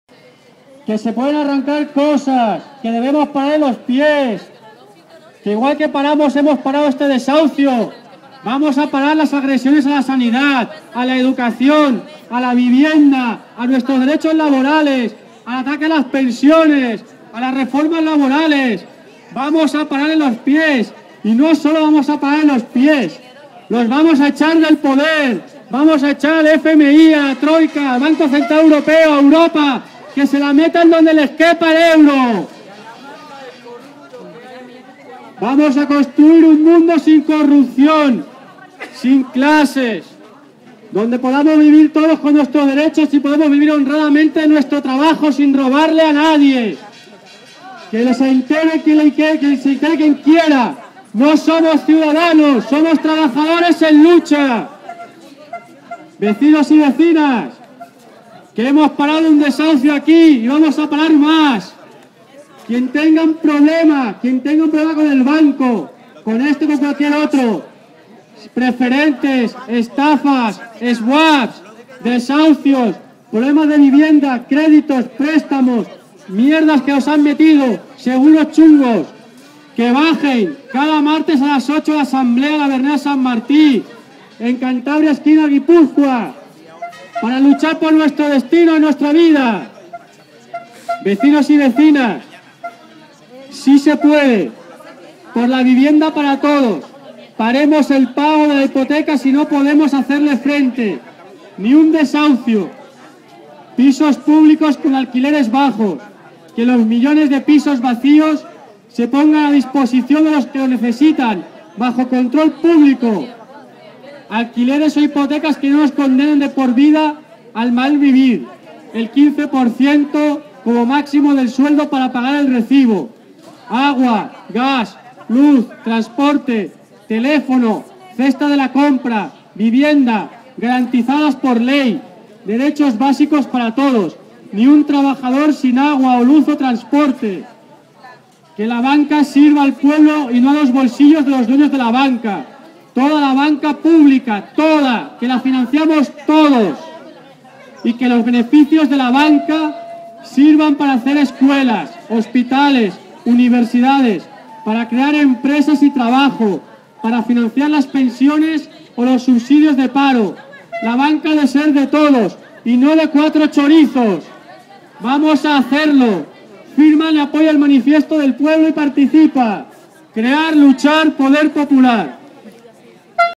L'Assemblea arrenca una reunió amb el director de zona de CX
Aquest divendres 10 de maig la pressió popular de l'Assemblea de La Verneda-Sant Martí ha aconseguit arrencar una reunió amb el director de zona de Catalunya Caixa per tal d'aturar el desnonament d'una família del Besòs.